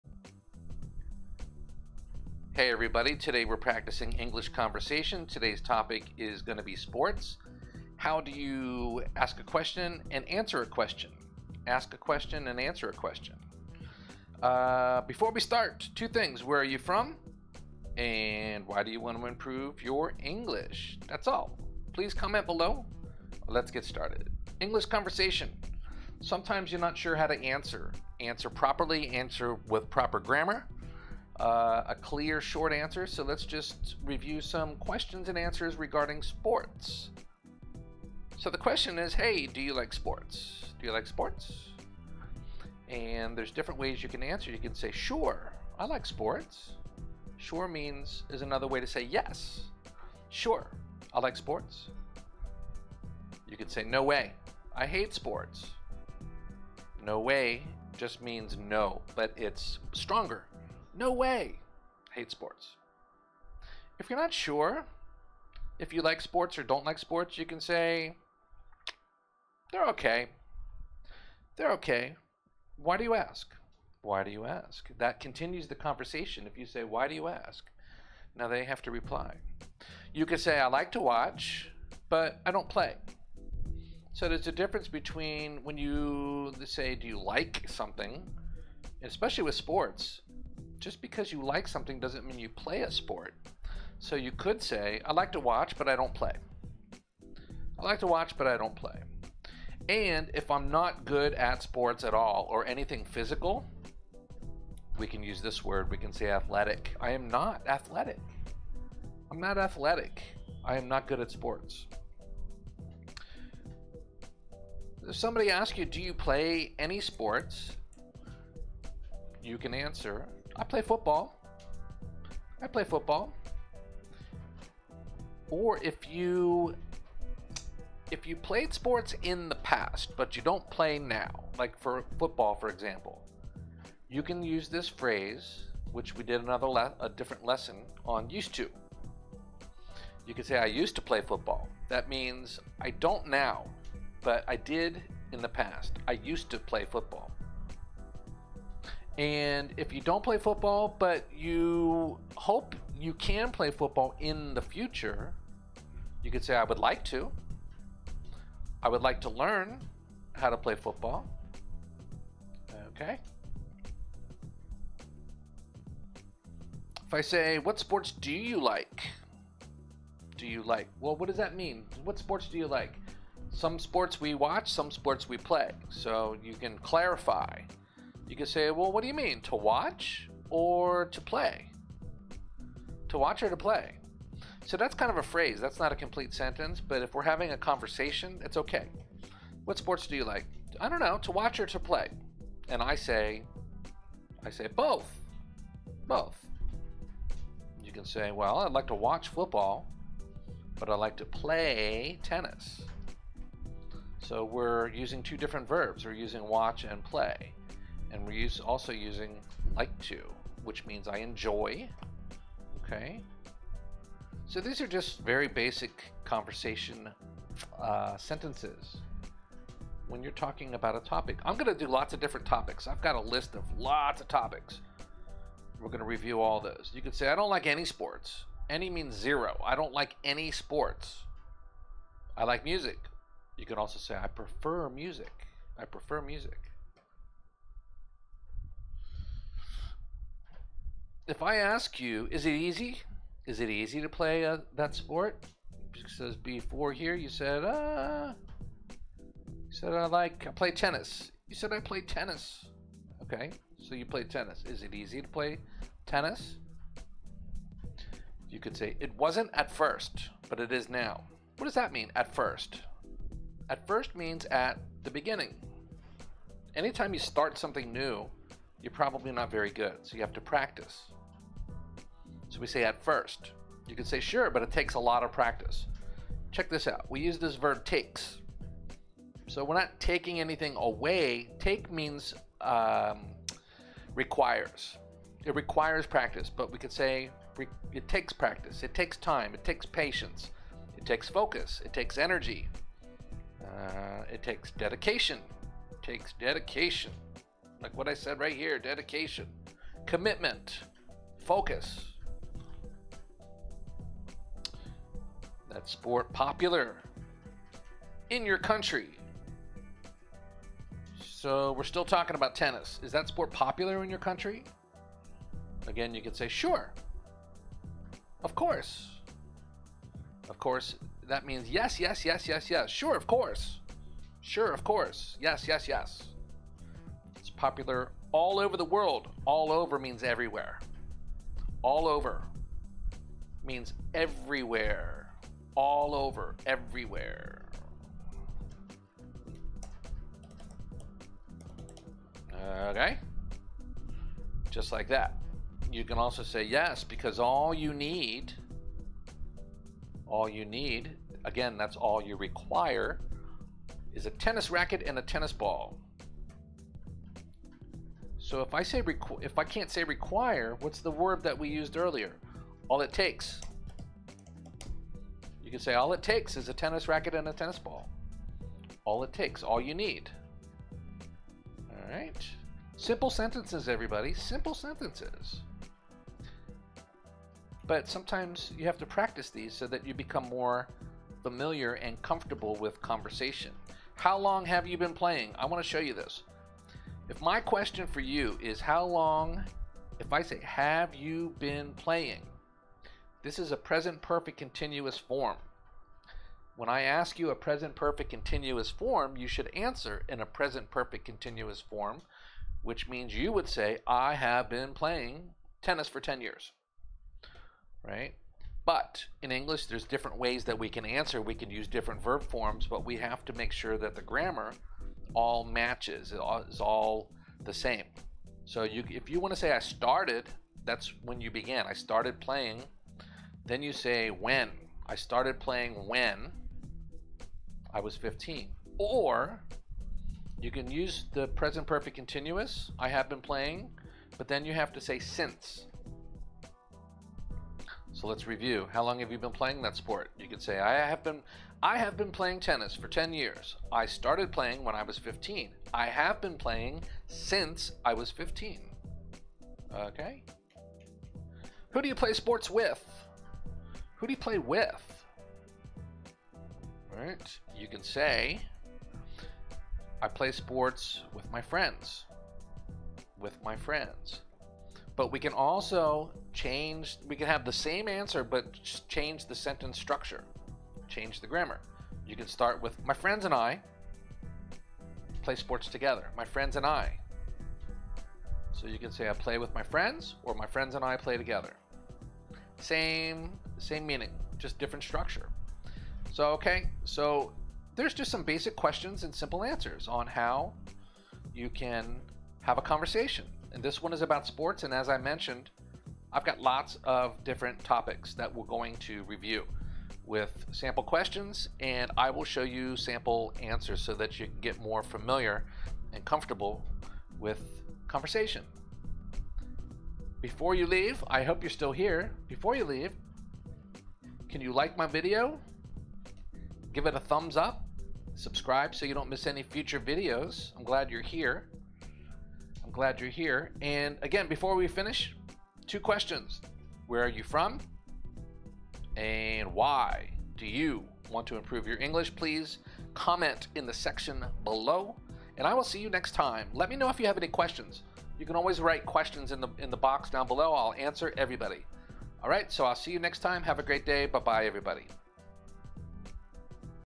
Daily English Conversation Practice for Beginners - Topic: Sports
This video is about Daily English Conversation Practice for Beginners, English grammar, pronunciation, vocabulary, listening and speaking. What you need to learn. Watch this video until the end. Hope you will like this video.